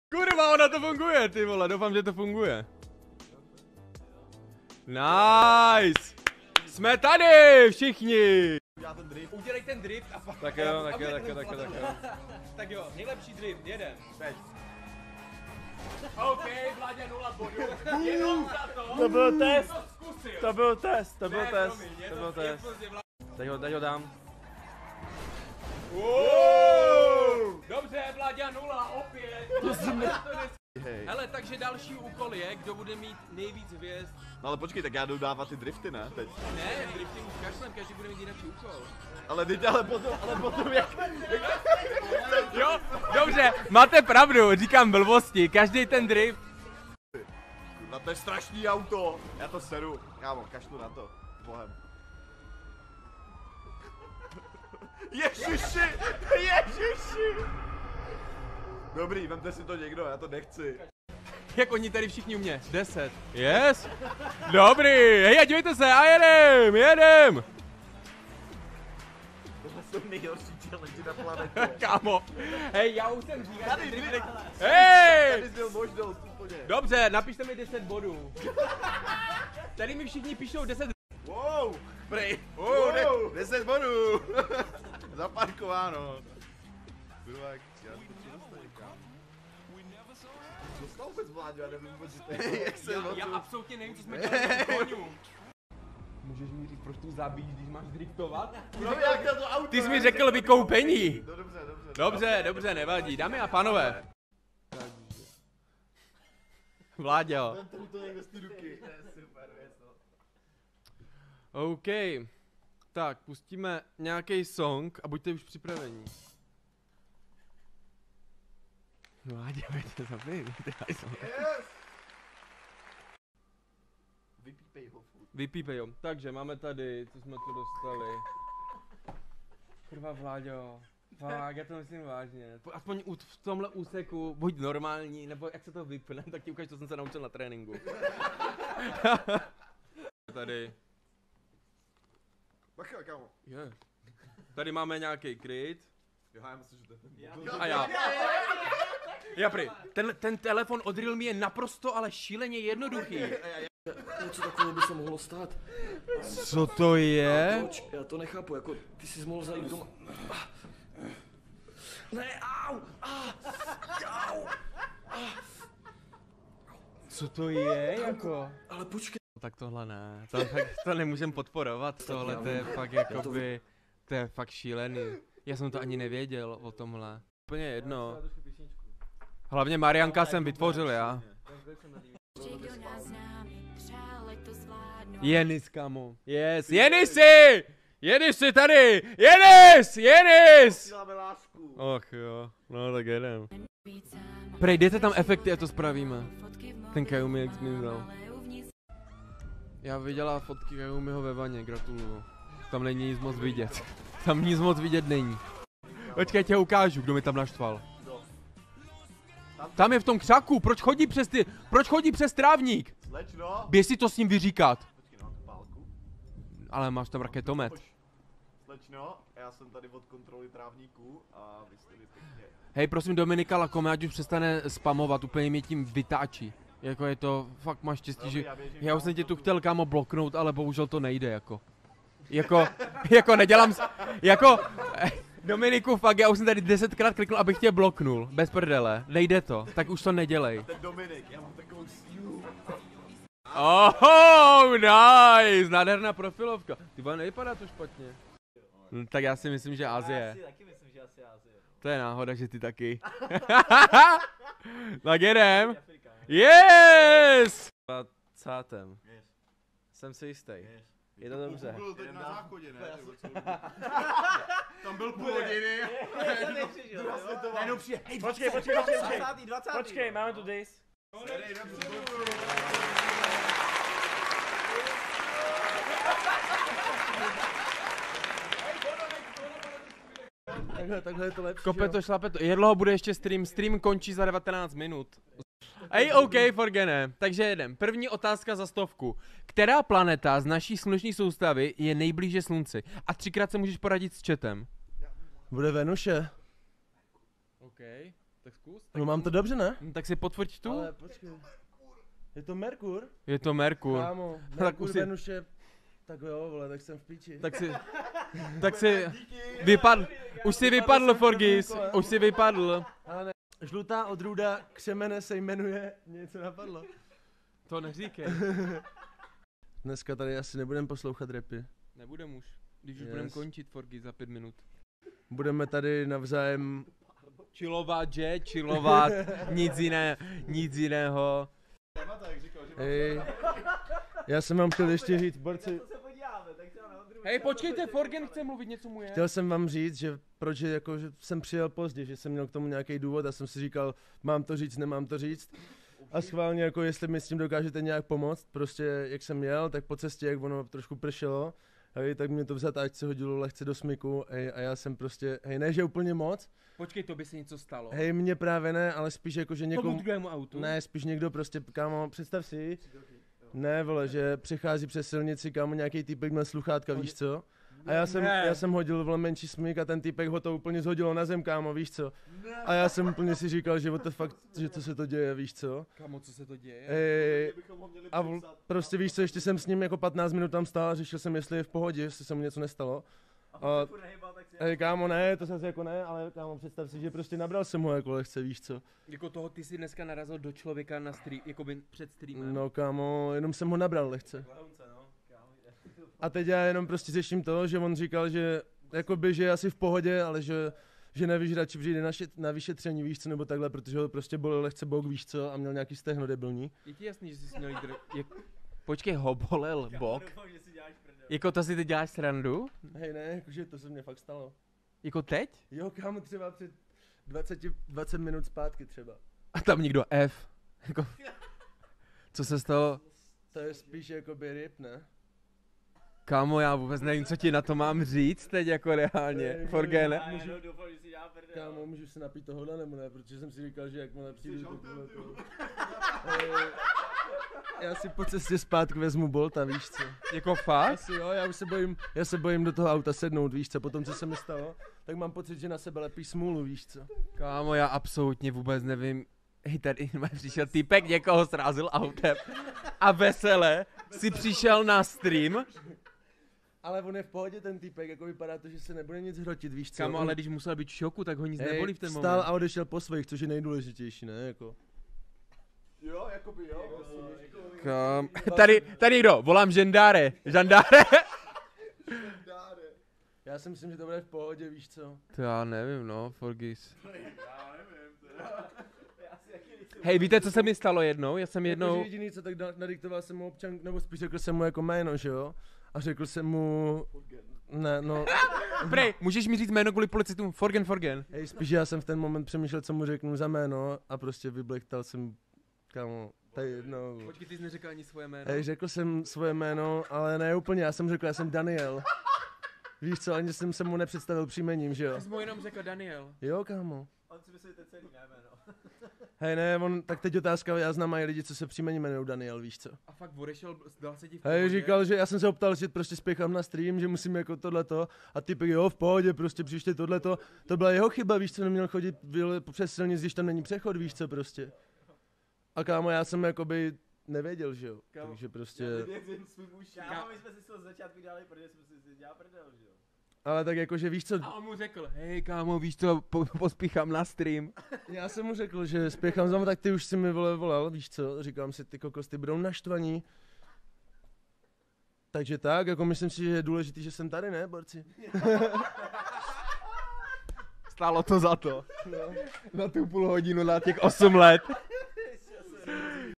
MŮJ PRVNÍ STREAM V ŽIVOTĚ ! w/ Práža, Vidrail, Forgen, Vláďa - Highlight
Ahoj.
Zdravím tě.
Toto video je vystřih z toho nejlepšího z livestreamu

Toto video je seznámení s Channelem

a tomhle kanálu bych chtěl aby ses tu bavil ...
Relaxuj a užívej...